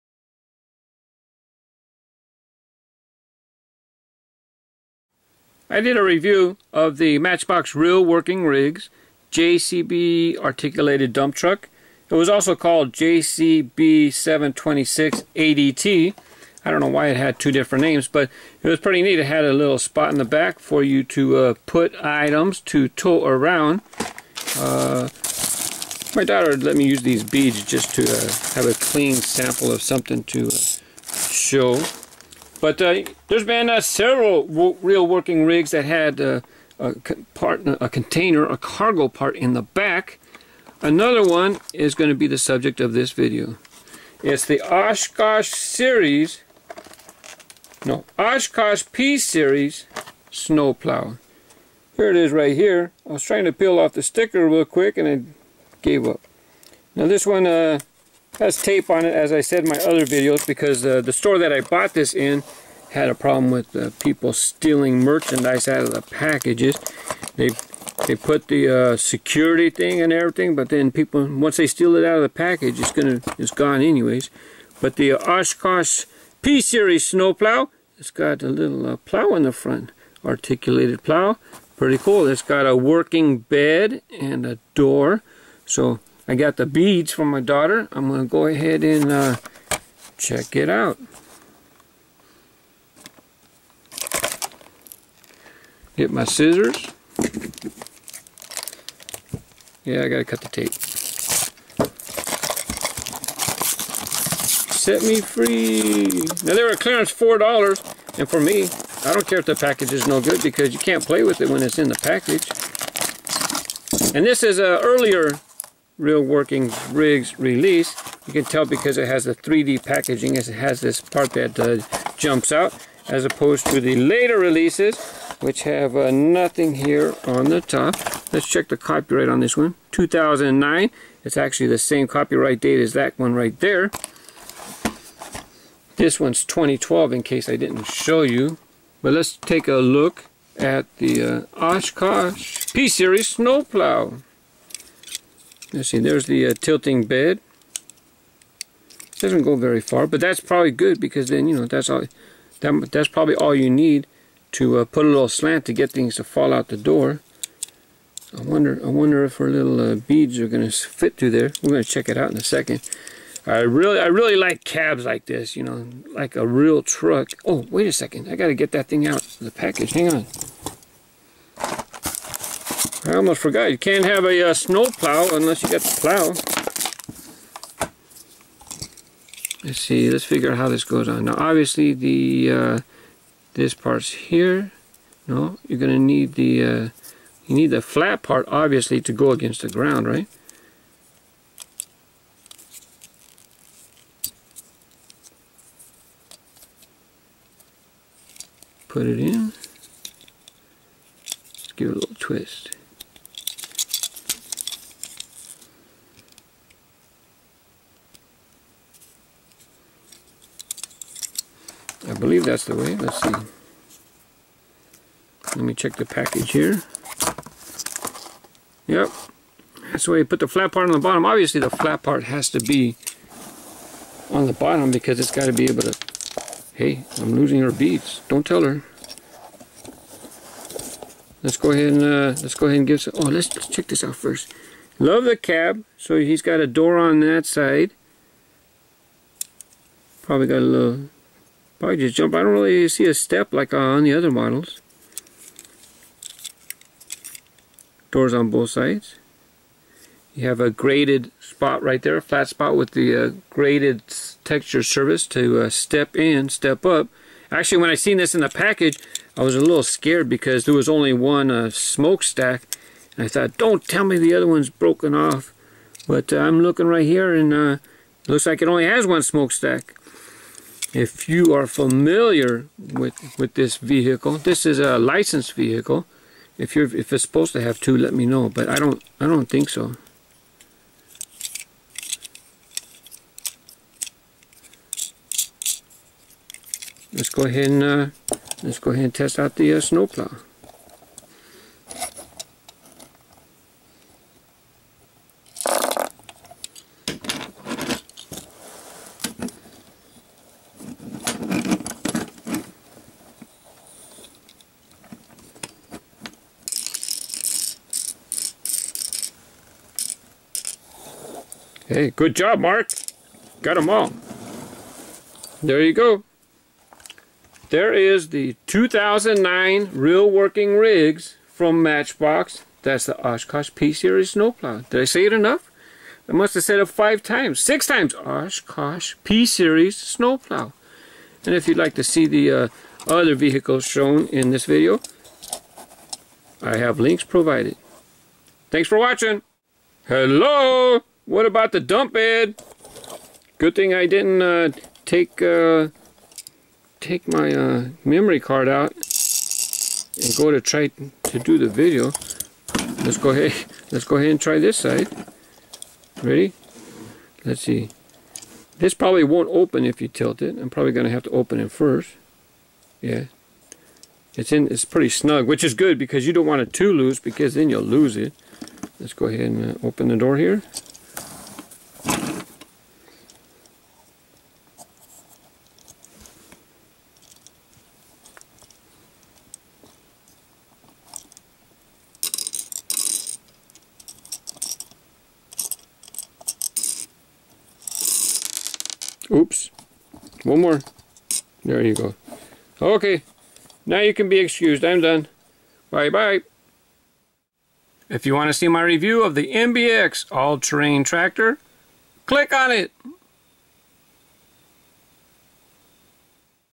Oshkosh P-Series Snow Plow Matchbox RWR, Matchbox Real Working Rigs
One of my favorite features on a Real Working Rigs model is a dumping feature. The Oshkosh is the orange one on the right and it has a dumping bucket in the back just like the JCB Articulated Dump Truck in yellow.